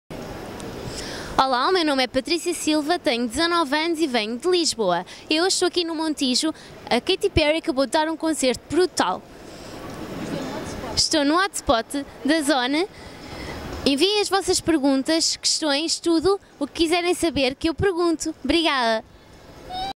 Patricia Silva - Cabine de Treino ZON@Fon - Fórum Montijo
Antenas no ar que o casting está a começar e os treinos na Cabine ZON@Fon para o MTV VJ Casting estão a aquecer. e vota no teu favorito. Se és cliente ZON regista-te e navega grátis fora de casa.
Sabe mais em zon/zonfon.pt